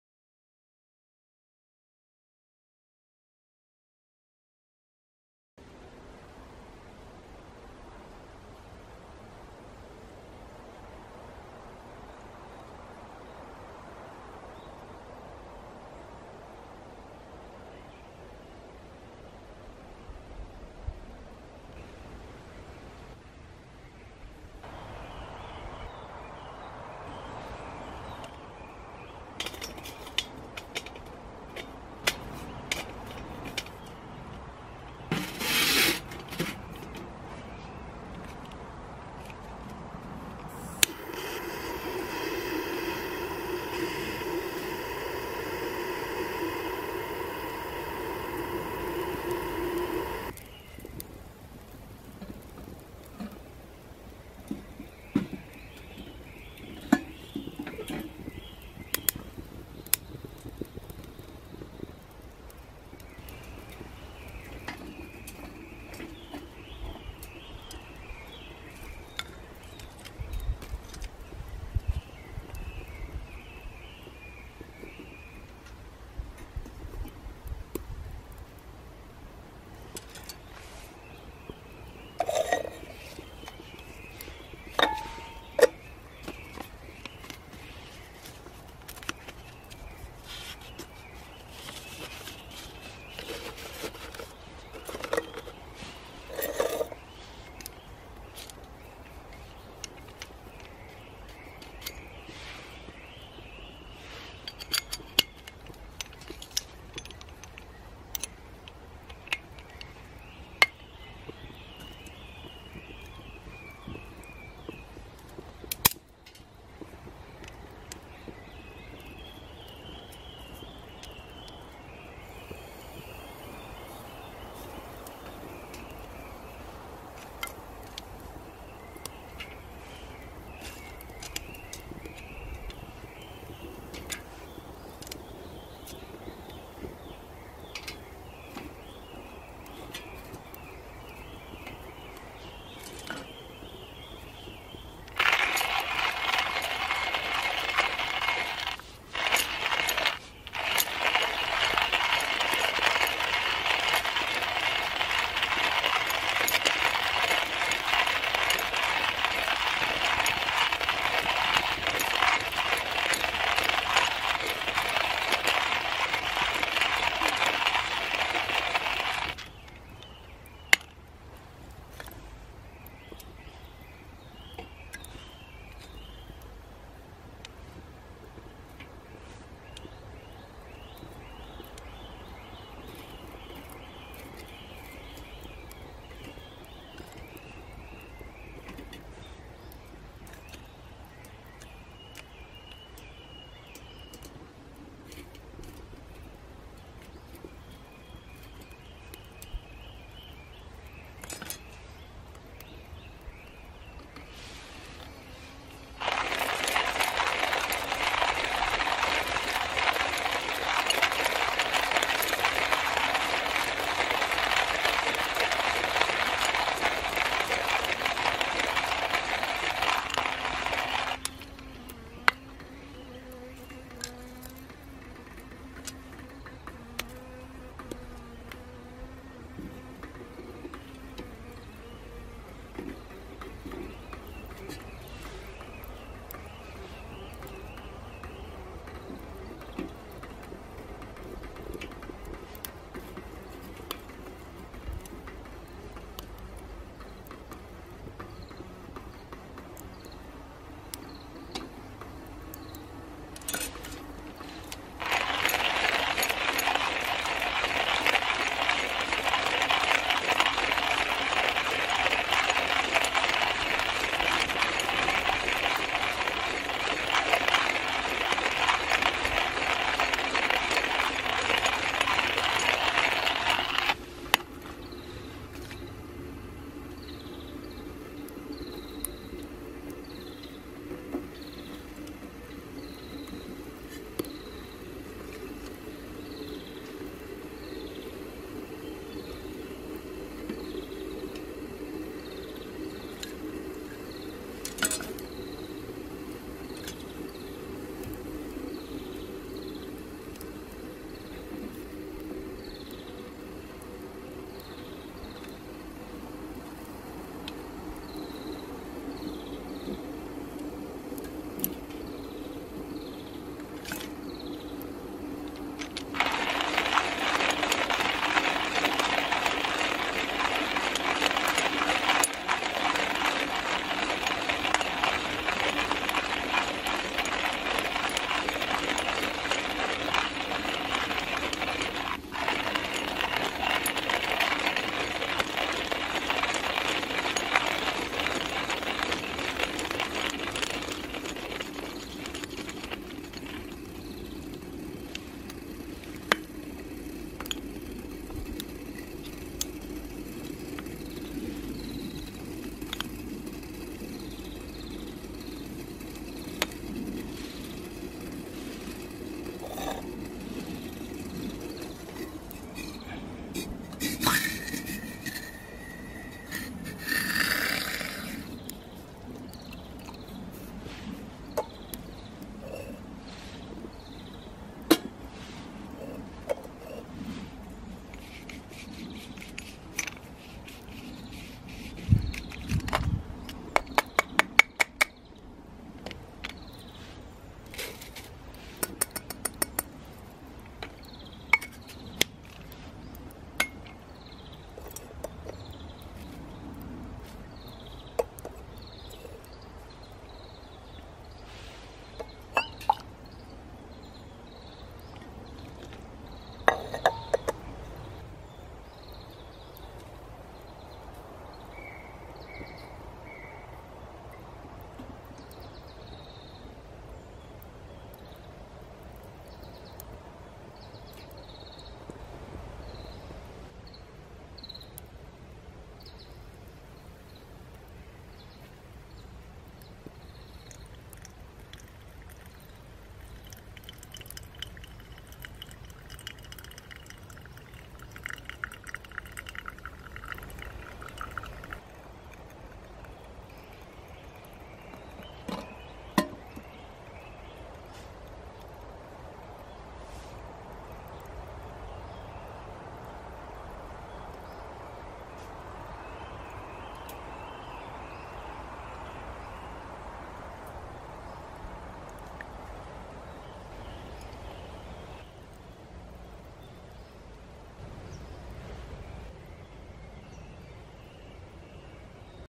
空き缶ウッドストーブで庭先コーヒー：2018年4月23日
新緑の爽やかな季節になりました。庭先のほっとスペースでウッドストーブで湯沸かししてコーヒーをドリップしていくだけの動画です。

【日々、備えながら楽しみ 楽しみながら備える】
by ゆとりありすと